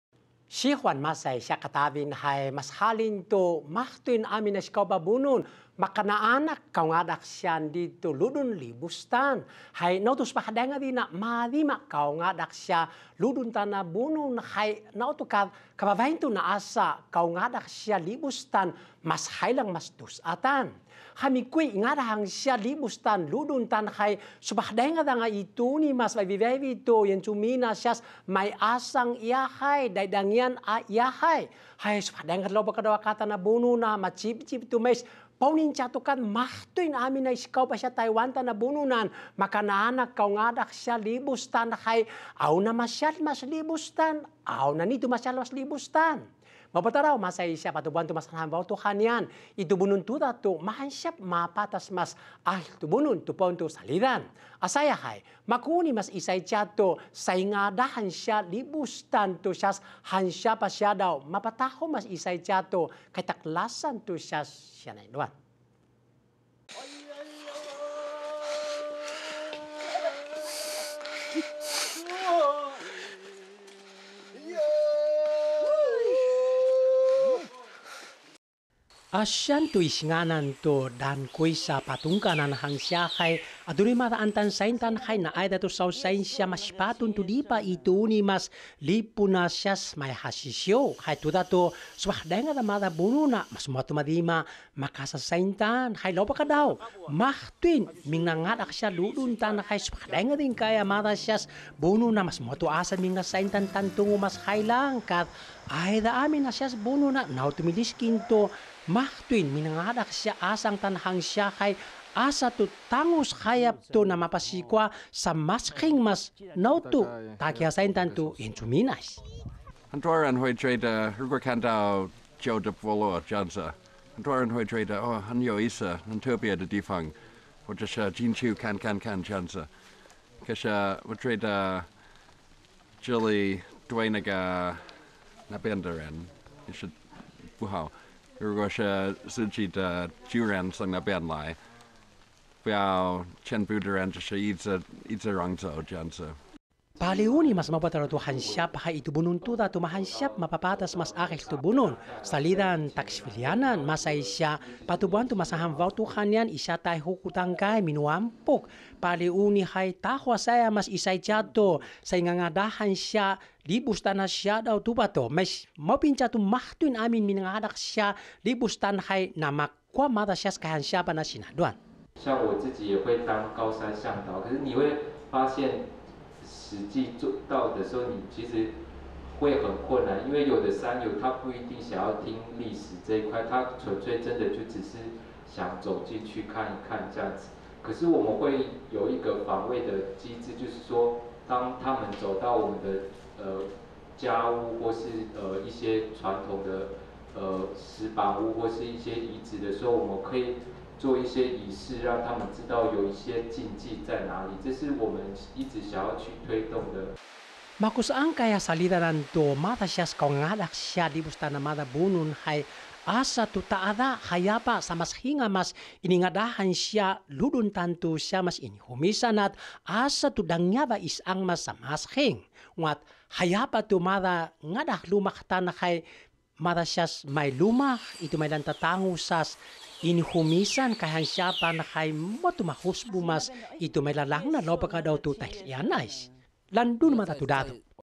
山林解禁之影響? 布農作家.嚮導分享看法 2020-04-23 Bunun IPCF-TITV 原文會 原視族語新聞
Na ni in kinsaun mais kaungadah ludun tu sinaduan

政府去年宣告山林解禁，許多山友迫不及待，想見識台灣山林之美，但許多山域，還存有不同族群的舊部落遺址，族人擔憂山林開放後，帶來的是危機或轉機？4月18日布農族作家Salizan，就以自身從事登山嚮導經驗，與大家分享看法。

這條知名的八通關越嶺古道，由於沿線有40多個日本駐在所，也成為許多登山客嚮往的探索路線。

開放登山管制，許多山友迫不及待一窺未知的山林世界，但也有登山客警覺到，山林解禁後的第一個課題，是要學會尊重當地的主人。

針對同樣議題，布農族作家Salizan Takisvilainan，4月18日在台北市一場座談會，以自身從事登山嚮導的經驗，分享開放山林下的危機與轉機。

Salizan希望，山友除了能夠傾聽這片土地下的歷史記憶，也可以多一分尊重，理解山林中的Mai lumaq 老家對於後代族人的重要性。